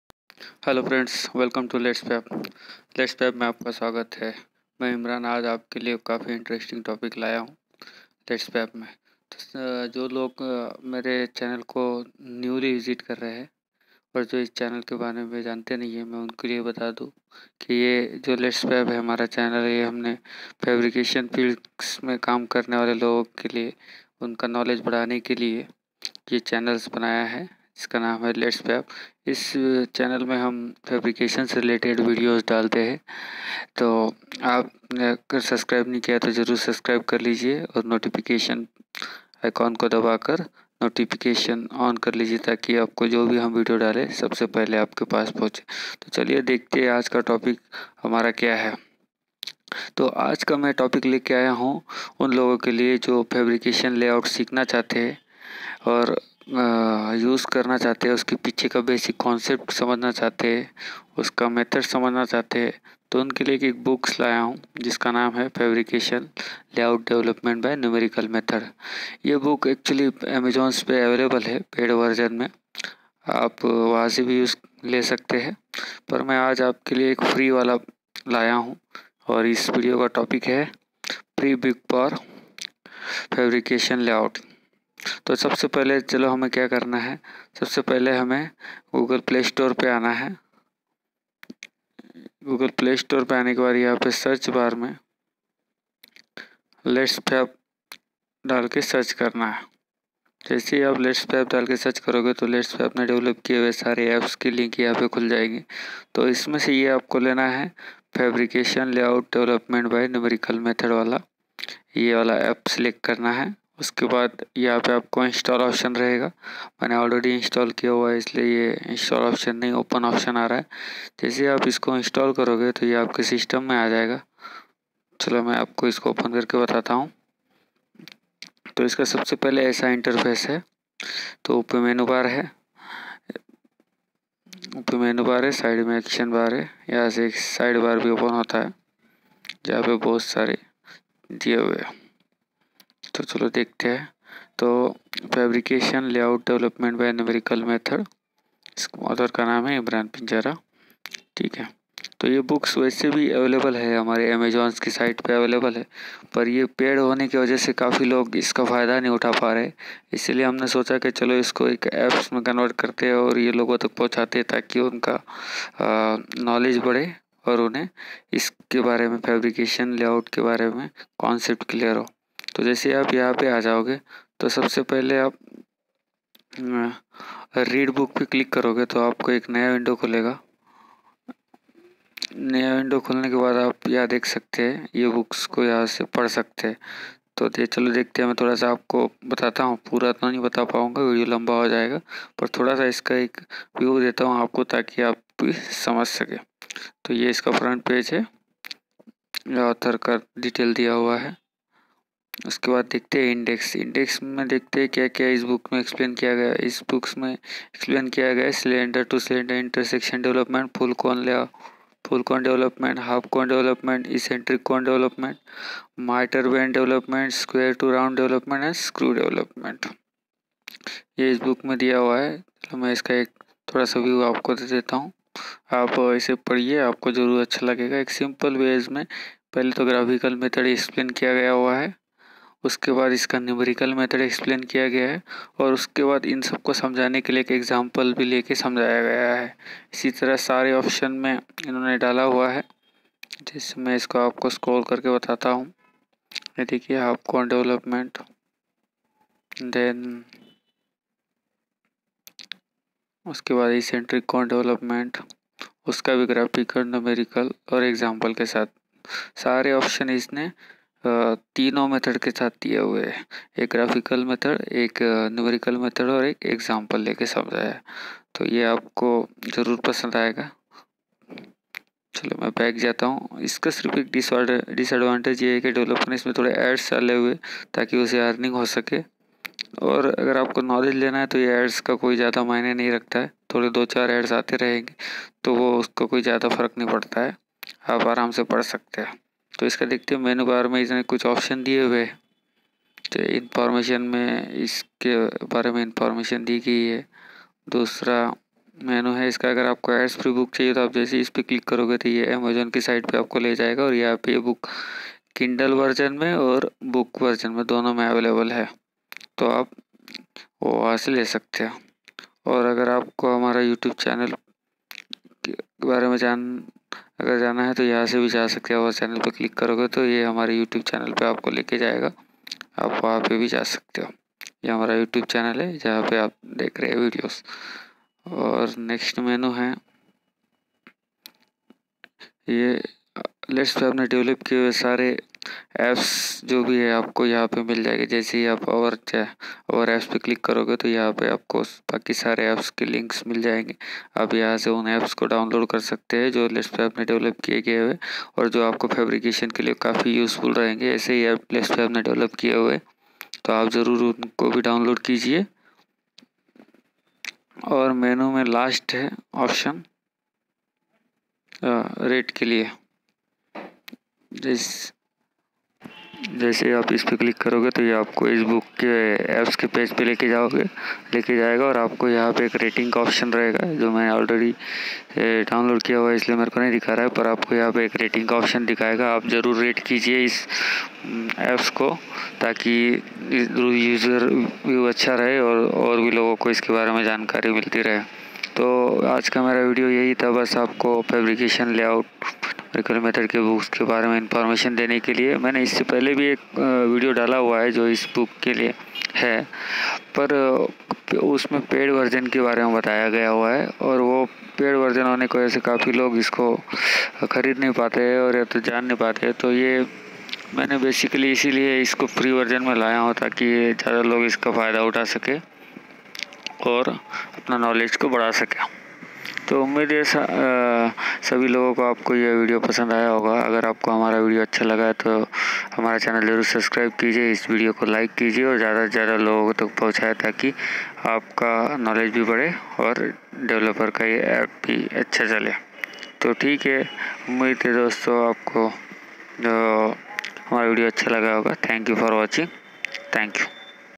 Free Book for Fabrication layout
This video is very useful for those who are looking for learning fabrication layout development.

Link for Mobile App:
Fabrication Layout book App:

Link for e-book for fabrication Layout development.